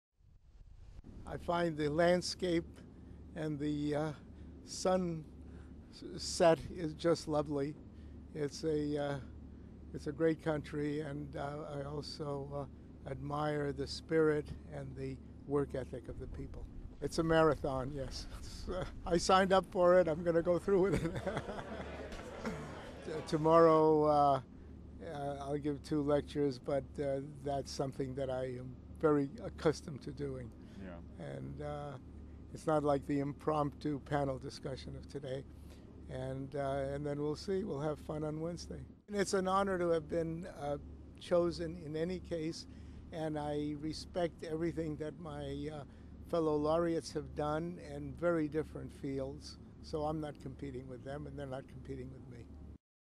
Millennium Prize Laureate Andrew Viterbi in Finland
The 2008 Millennium Prize Laureates came to Finland for the Millennium Technology Week in June.